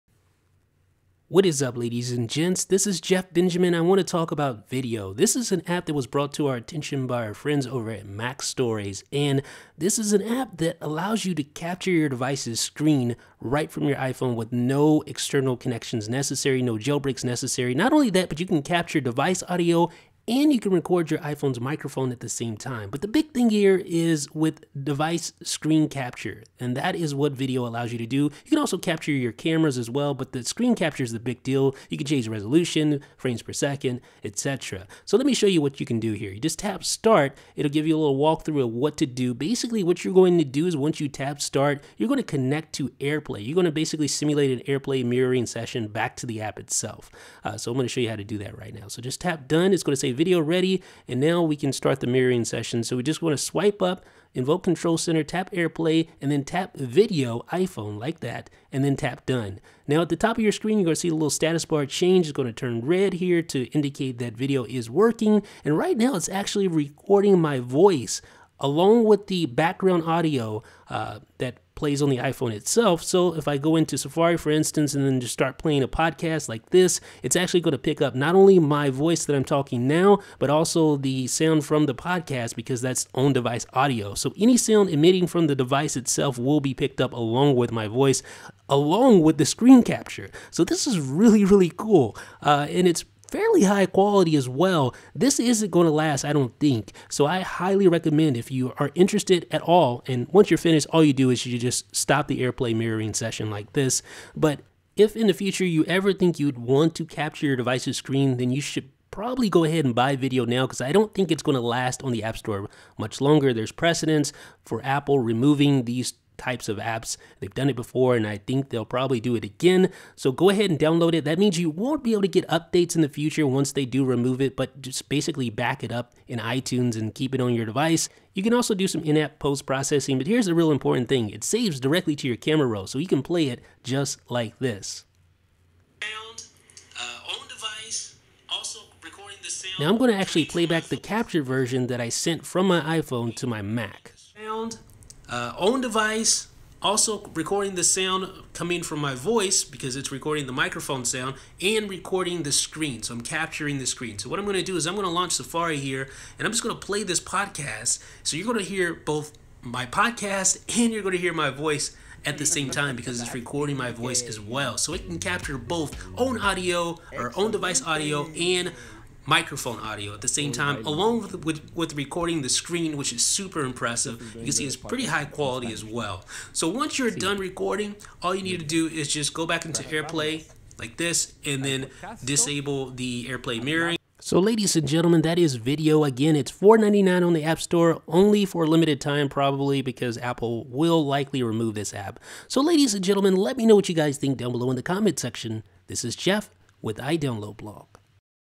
Vidyo: screen recording on iOS without jailbreaking!
Update: Vidyo! has been removed from the App Store.

Vidyo is a new iOS App Store app that allows you to capture your screen, microphone, and device audio, no jailbreak required.

About iDB